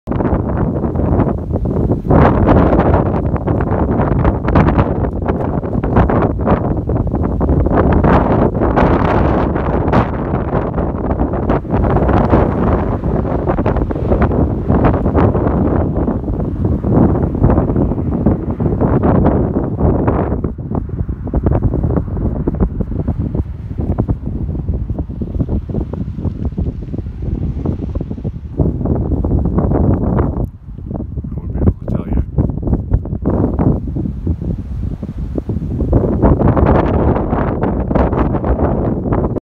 Odd thing floating in the distance
Could be a bag or something, but it’s fun to speculate.